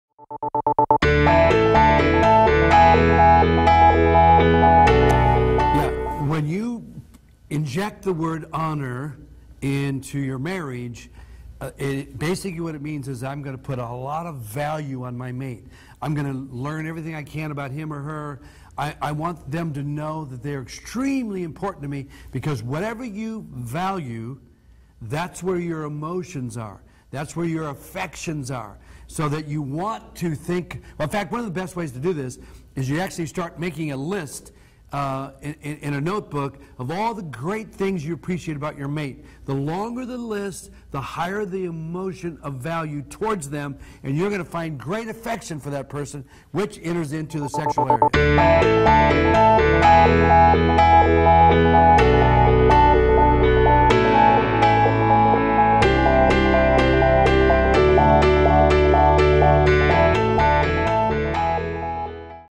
What does the word honor mean in a marriage? Dr. Gary Smalley and Ted Cunningham
Dr. Gary Smalley and Ted Cunningham- What does the word honour mean in a marriage? See Dr. Dave & North Americas top experts answer all your marriage and family questions